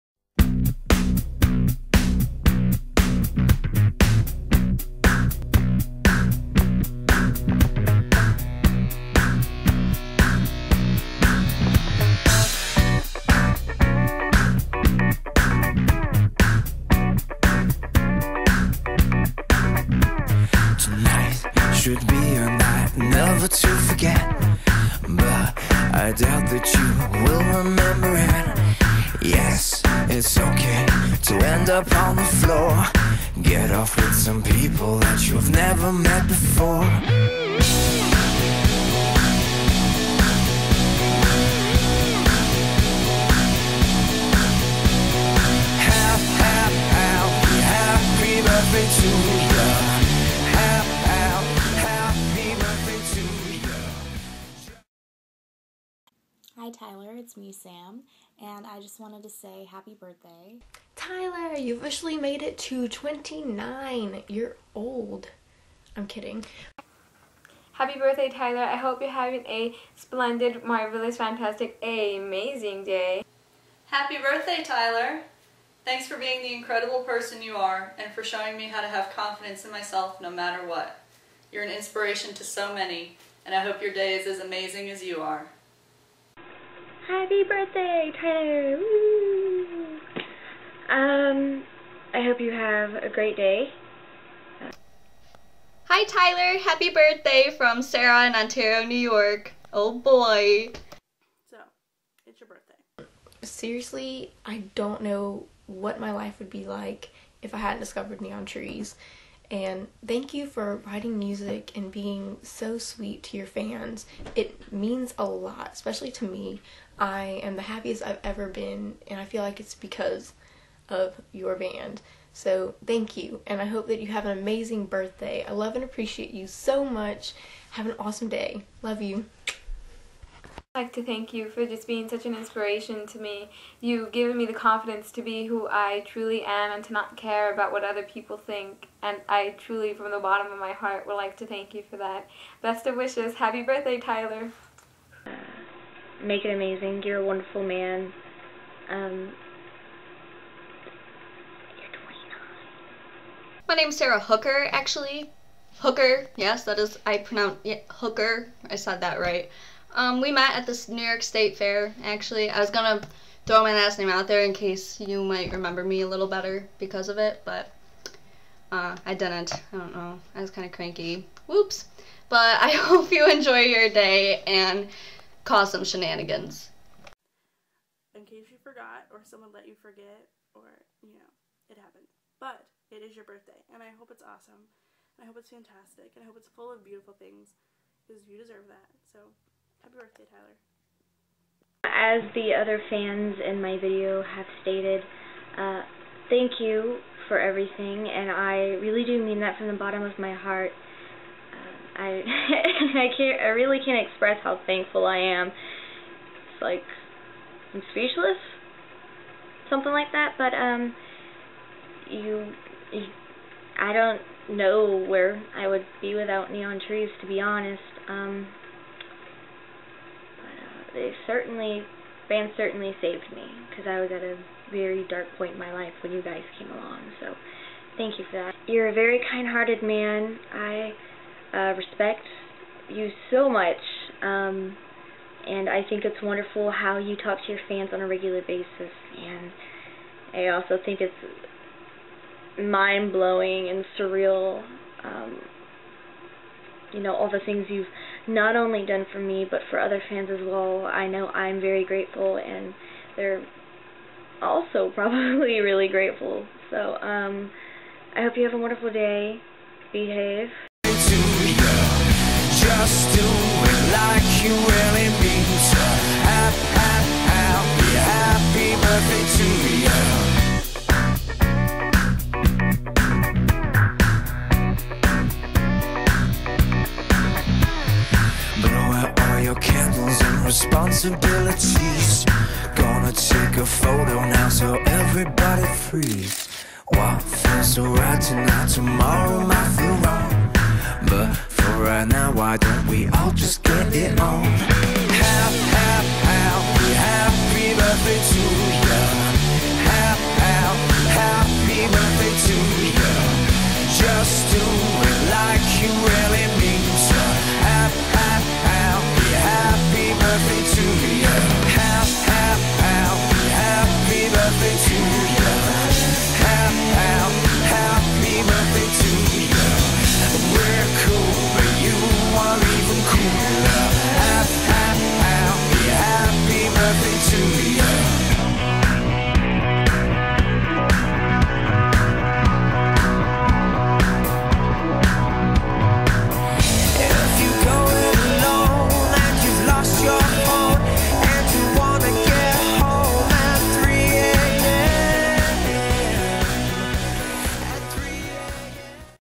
Happy 29th birthday, Tyler Glenn!
A video I put a lot of time and effort into for Tyler Glenn, the frontman to the incredible band Neon Trees!
Thank you to everyone who was in this video. It means so much. ENJOY!

Thanks Sara Pina, Jennifer Hambro, Charleigh Lowe, Kayla Longtin, Paige Menge, Amber Lajara, Keyla Sanchez, Sarah Hooker, Morgan Magnarella, Danielle Dennison, Millie Lawhorn,  Sam Reish, Maria Rella, and Angel Sinisgalli.